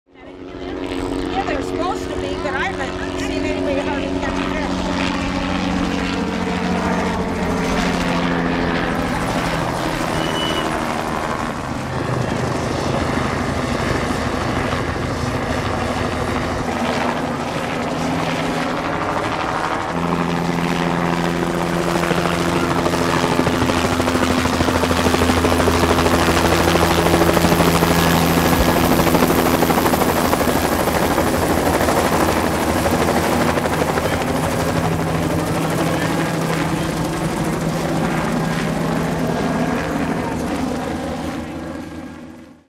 2022 National Night Out - Chicago Police Department Helicopter
A Chicago Police Department helicopter did a cool flyover at the 2022 National Night Out at Monee Reservoir.